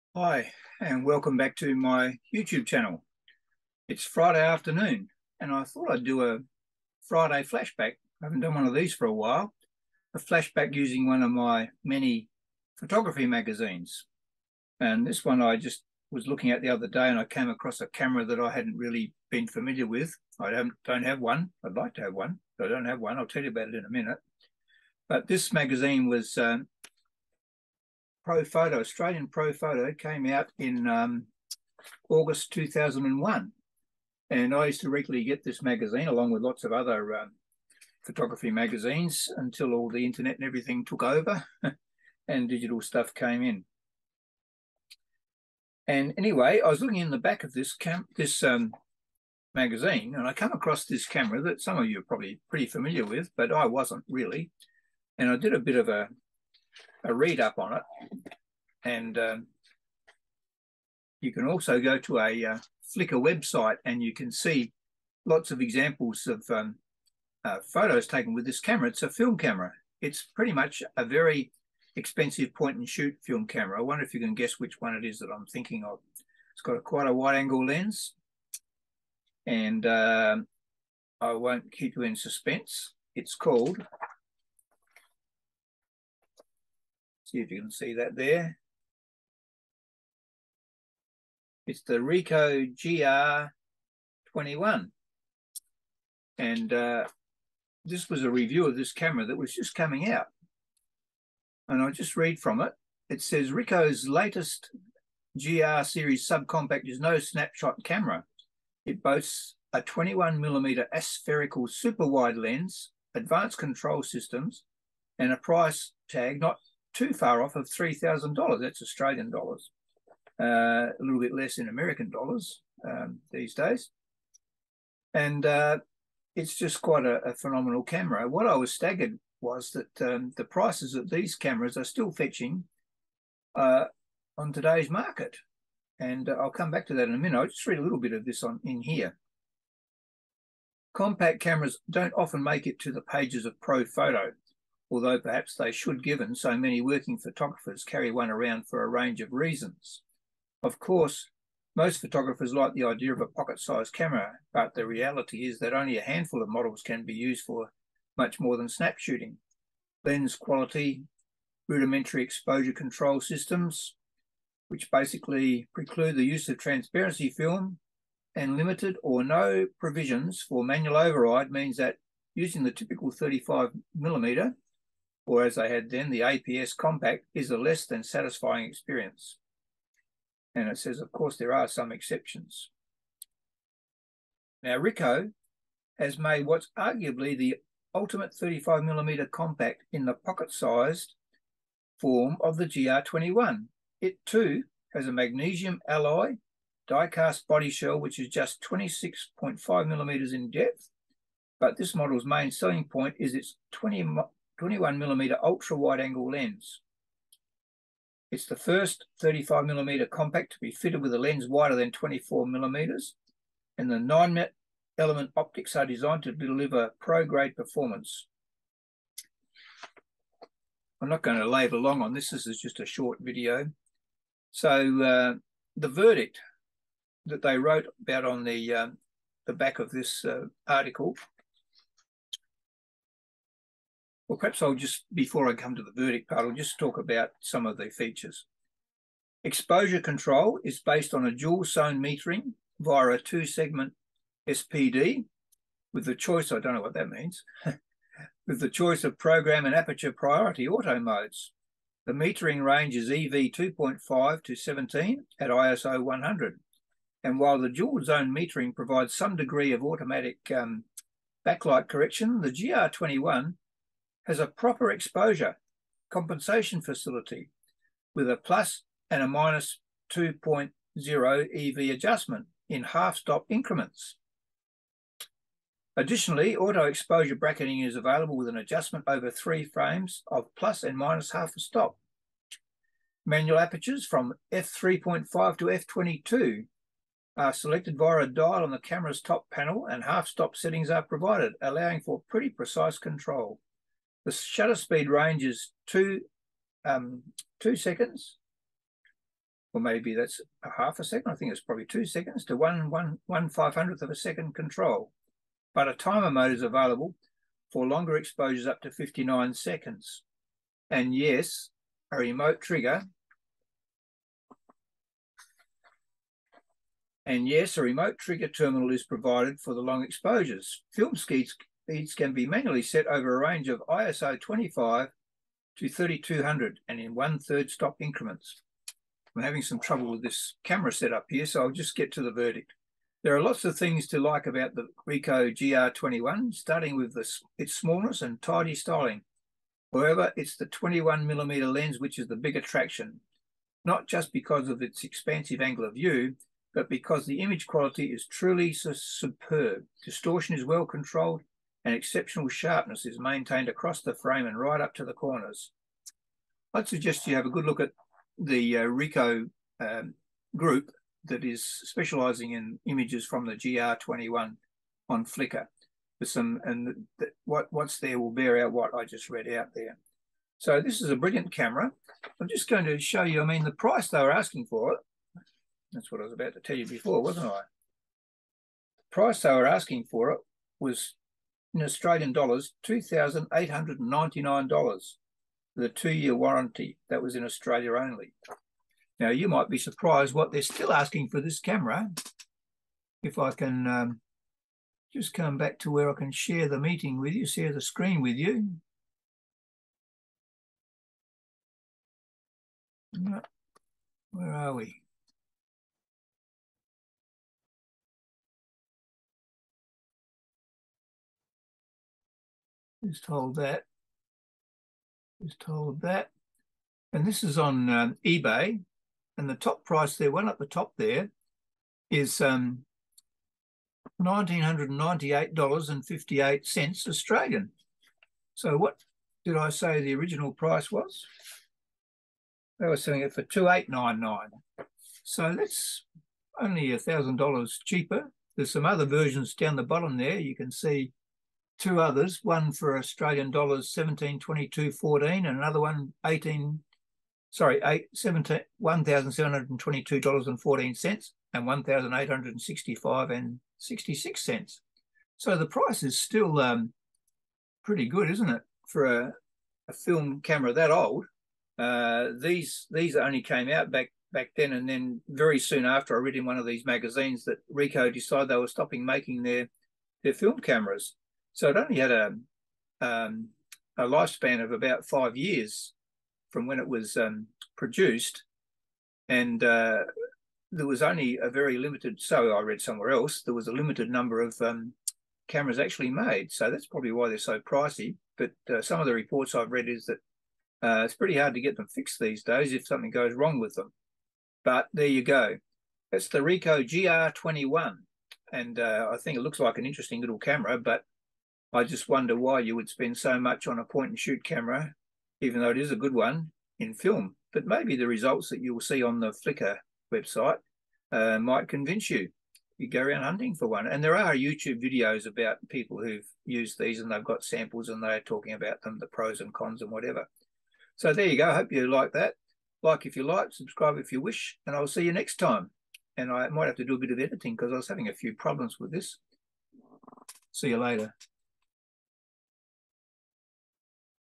Friday Magazine Flashback- The Ricoh GR21
In August 2001 ProPhoto announced the arrival of the Ricoh GR21. A very expensive point and shoot film camera. Did you buy one back then? Would you buy one now?
About 5 years later Ricoh stopped making them.
Here is a link to Flickr where you will find some images taken with this camera.
Specifications
    Lens: GR Lens 21mm f/3.5 (9 elements, 6 groups)
    Focusing: Autofocus multi-subject
    Shutter Speeds: Electronic shutter - 2 to 1/500 second.
    Exposure Control: Aperture Priority Mode
    Film Speeds: ISO 25 to ISO 3200 (DX)
    Batteries: 1xCR-2
    Size: 117mm x 64mm x 26.5mm (Grip: 38.5mm)
    Weight: 200gr